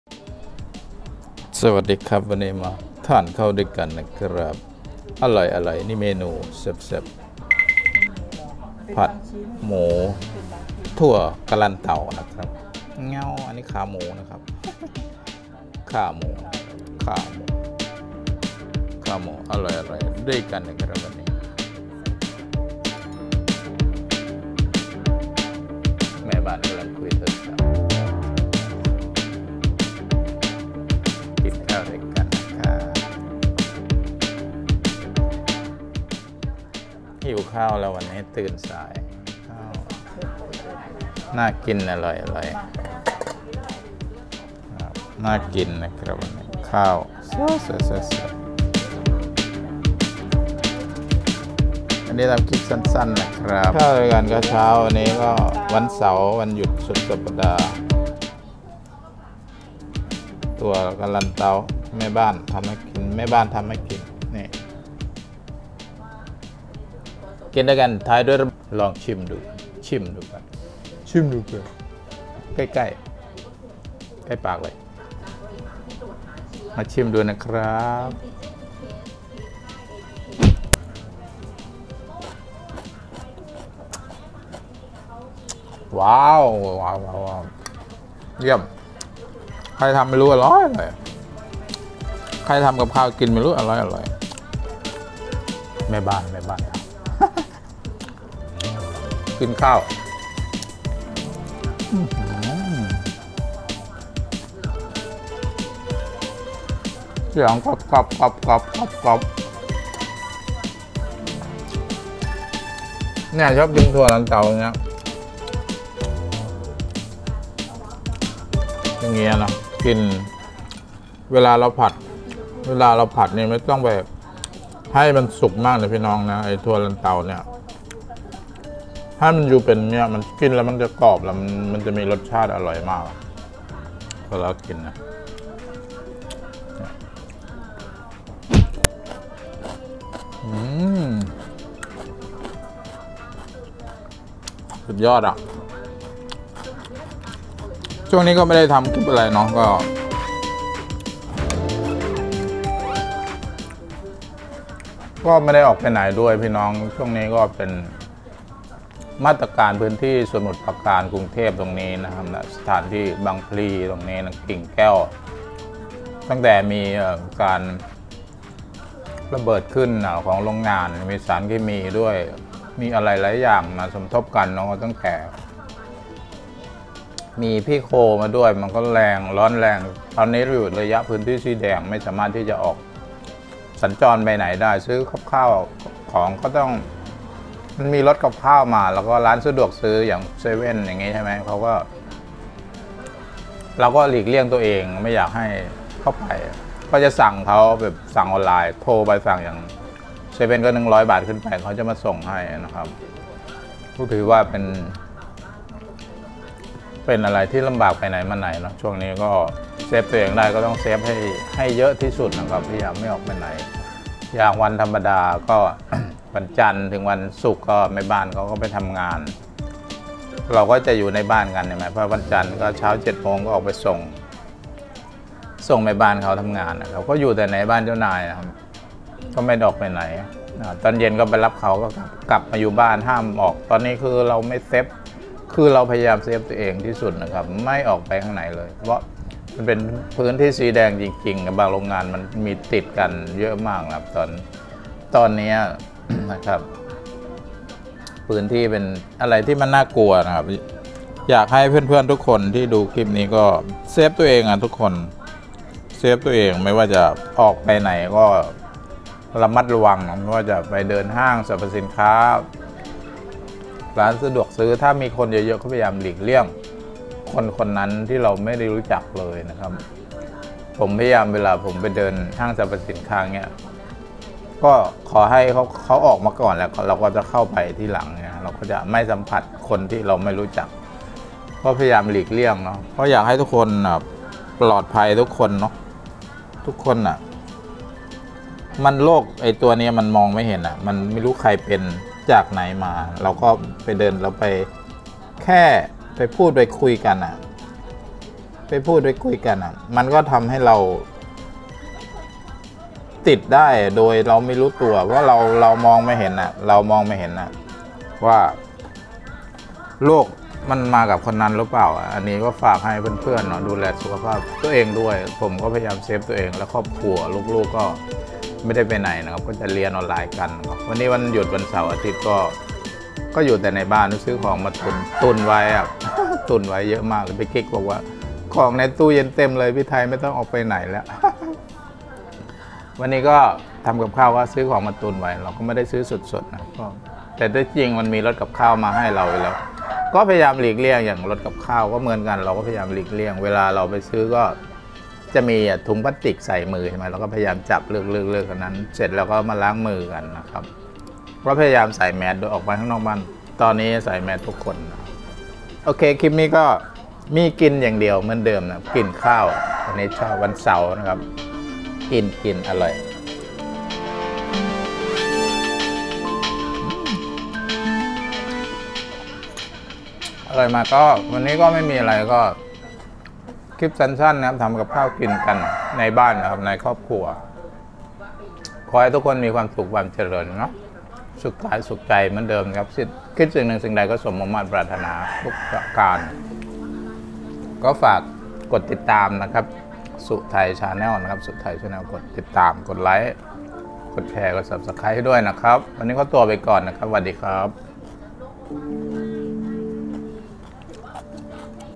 ผัดหมูถั่วกะลันเตาแซ่บอร่อย
มาทำกับข้าวผัดถั่วลันเตากินให้อร่อยนะครับ